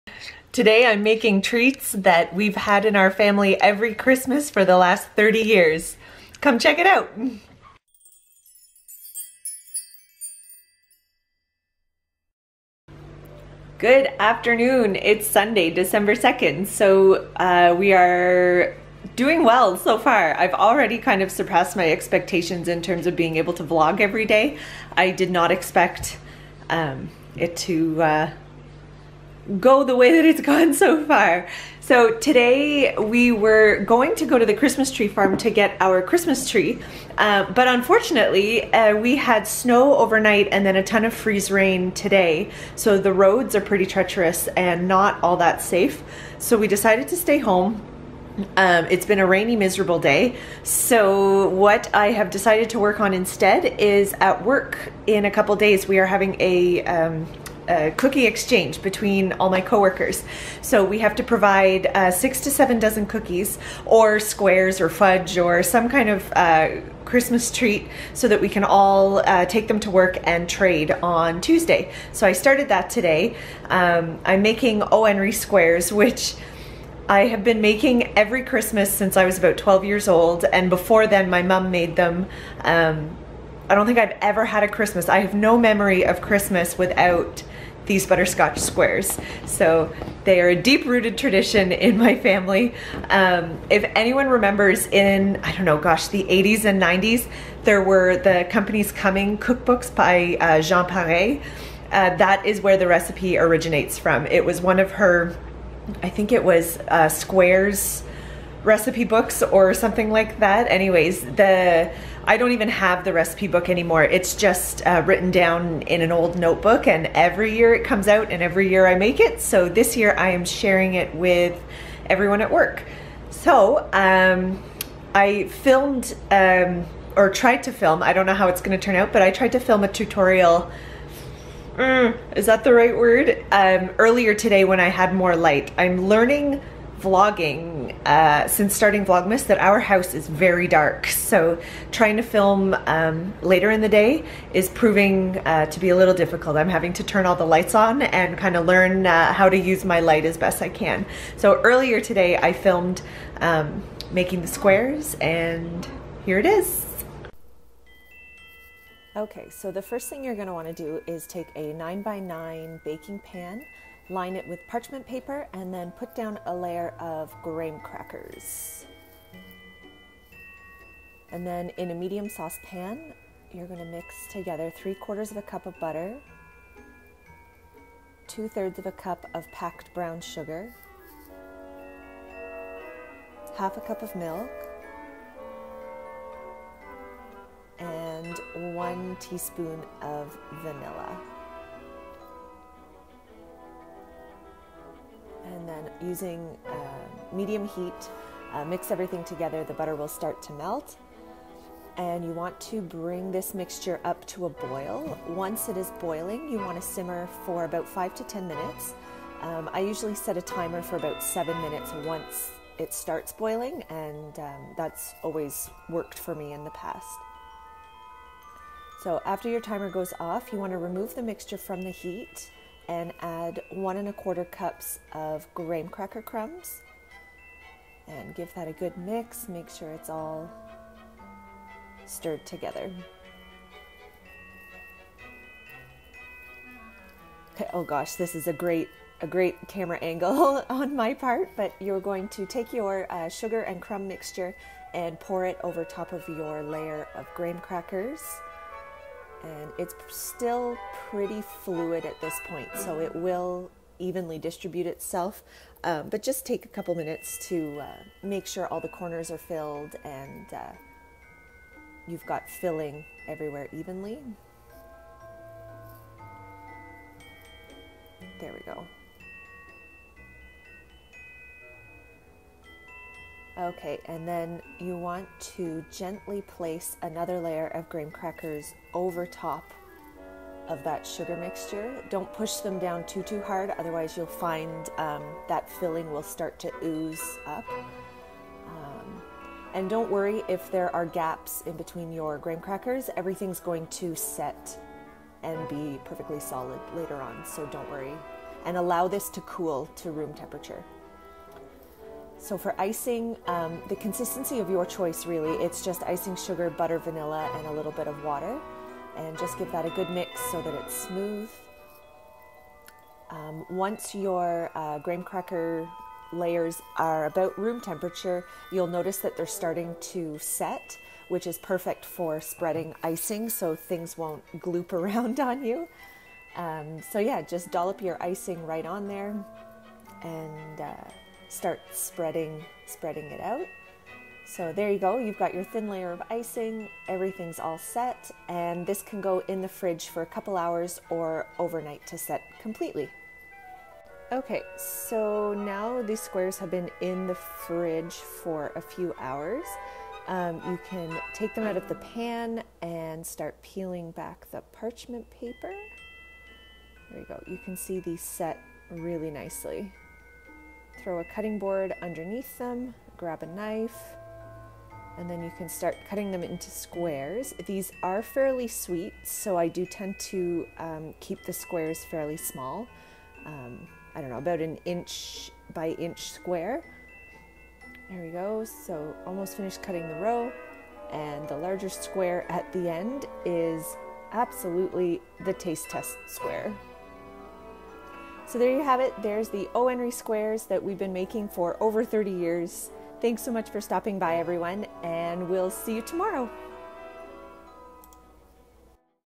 VLOGMAS 2018 - A Thirty Year Baking Tradition
Day 3 of Vlogmas and I challenged myself to try something new with my editing software. A Voiceover!
Needless to say the time spent on editing this video has taught me that I either need to learn to work faster. or maybe not put a voiceover on every vlog!

In Today's vlog I make Oh Henry Squares, something that has been in my family for over 30 years. The recipe originally came from Company's Coming recipe books by Jean Pare. I will post the recipe down below in the pinned comments.